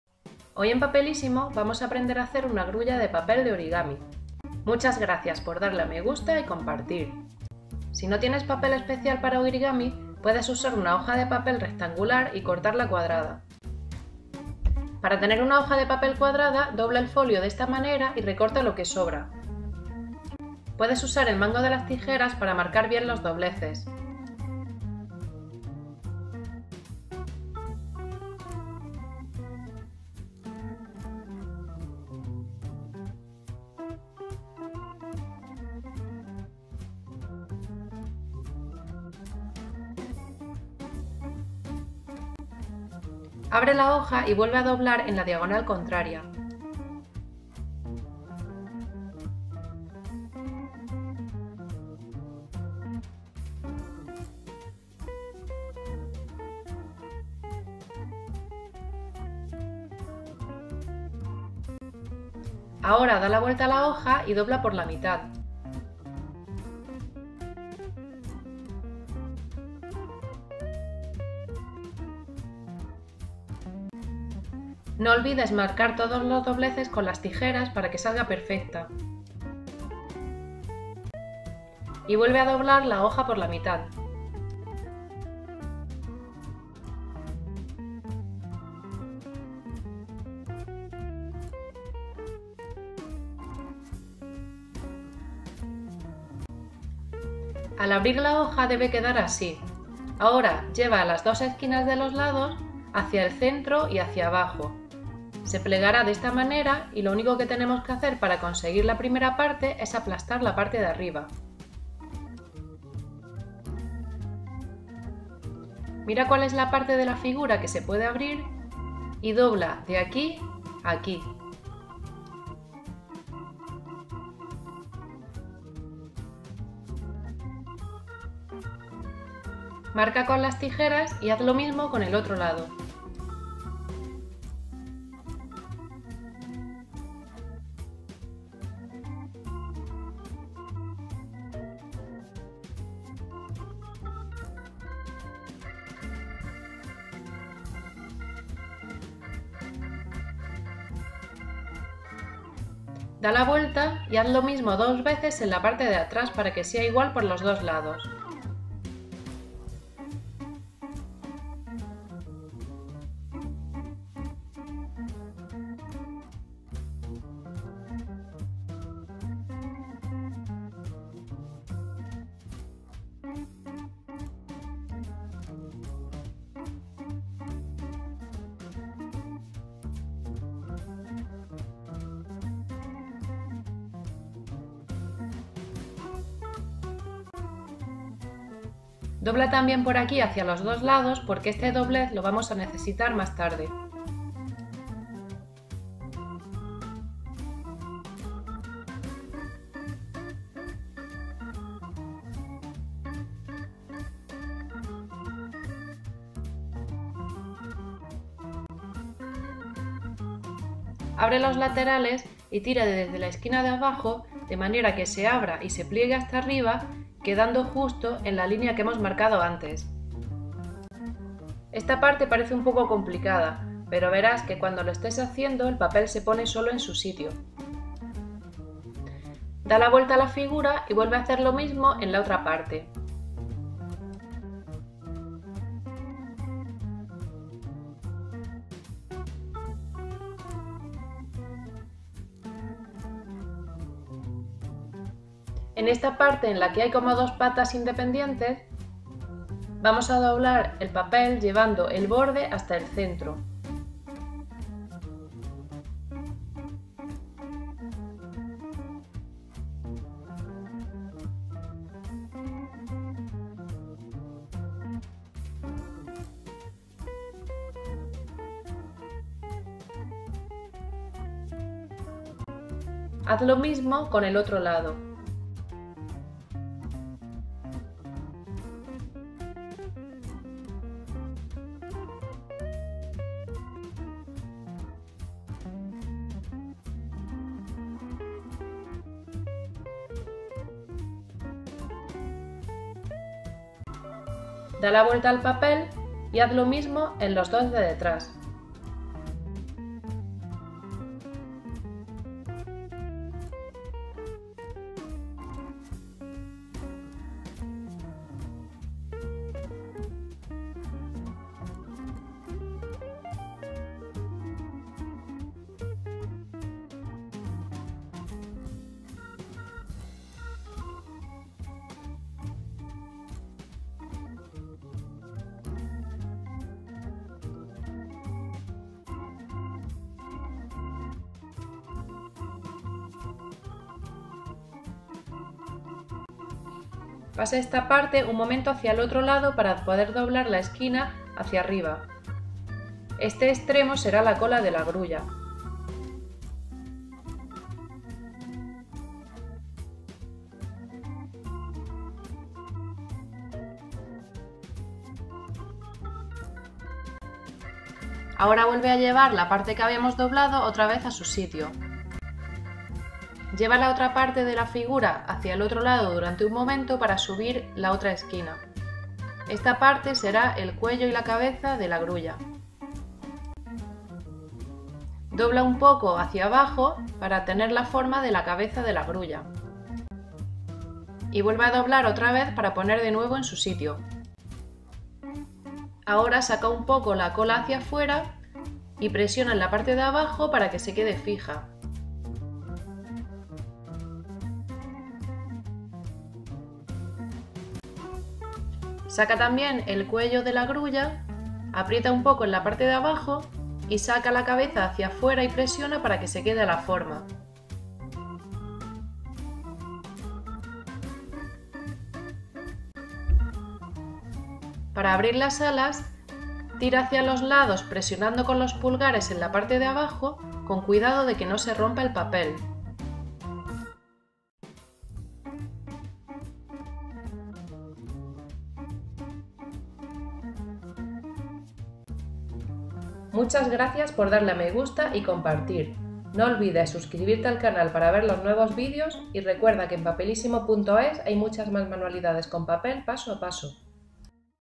Cómo hacer grulla de papel (origami - papiroflexia)
Tutorial paso a paso para hacer una grulla de papel muy fácil con papiroflexia.

Una de las formas clásicas del origami es la grulla de papel. Una figura llena de significado en la cultura japonesa (historia de las mil grullas de papel), y que ahora está de moda en la decoración DIY, bodas...

Según la leyenda japonesa, tu mayor deseo se hará realidad, construyendo mil grullas de papel. Esta tradición se remonta a la historia de Sadako, habitante de Hiroshima durante la Segunda Guerra Mundial.

Aprende a hacer grullas de papel siguiendo las intrucciones del vídeo.